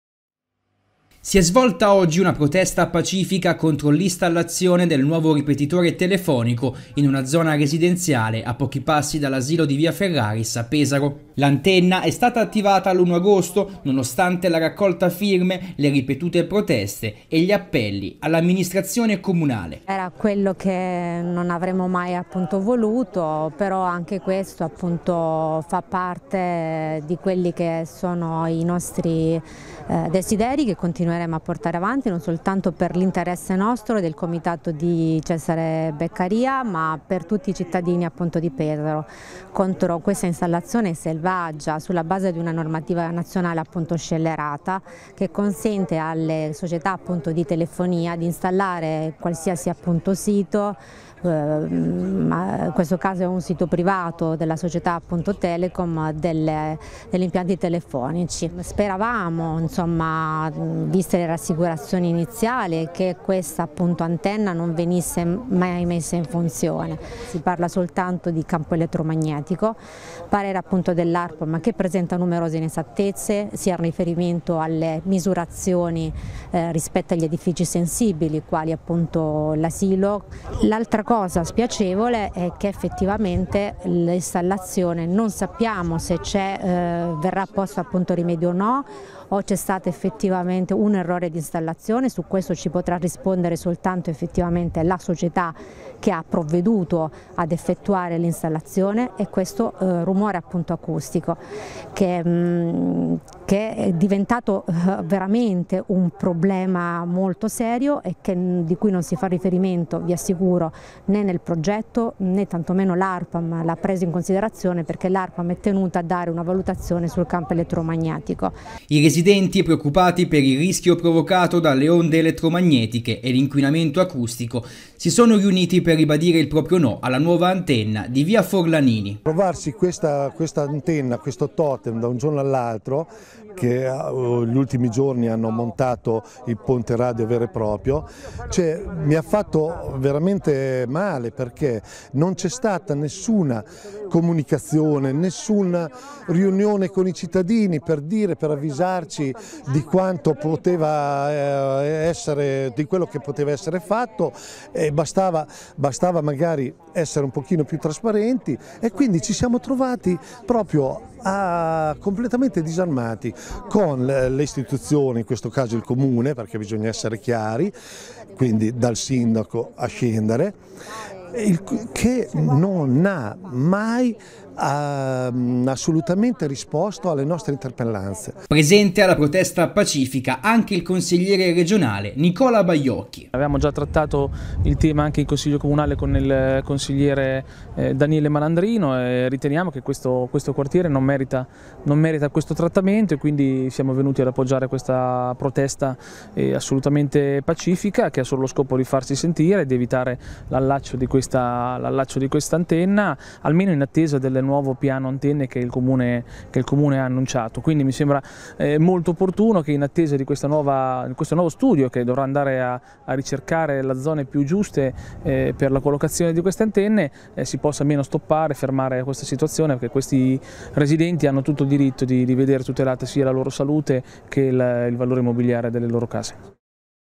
Nuovo ripetitore telefonico a Pesaro: residenti in protesta
Nuovo ripetitore telefonico in Via Forlanini a Pesaro. Scatta la protesta pacifica dei residenti.